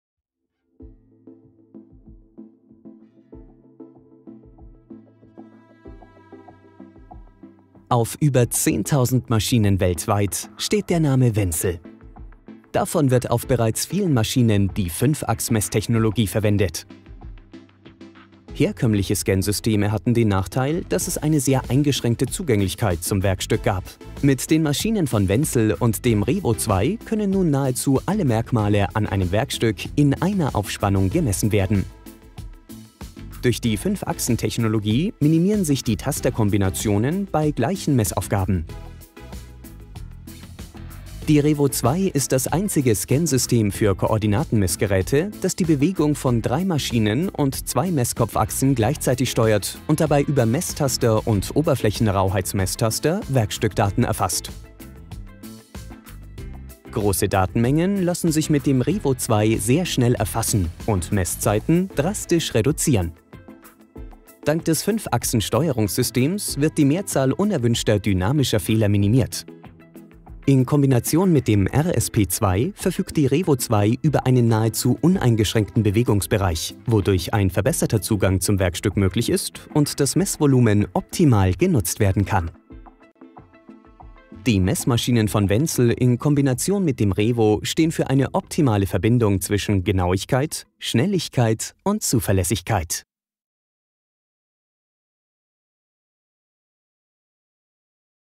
WENZEL LH Koordinatenmessgerät mit REVO 5-Achsen-Messsystem - Highspeed meets precision
Sehen Sie die erfolgreiche Kombination des 5-Achsen-Messsystems REVO von Renishaw auf einem Portalmessgerät der LH Baureihe an. In der neuen Version der WM | Quartis Messsoftware wurden weitere Verbesserungen zur Erhöhung der Genauigkeit und Benutzerfreundlichkeit implementiert. Der Revo Sweep-Scan erlaubt es Oberflächen schnell und präzise zu Scannen. Hierbei erfasst der Revo Tastkopf die Oberfläche durch eine schnelle Schwenkbewegung des Tasters.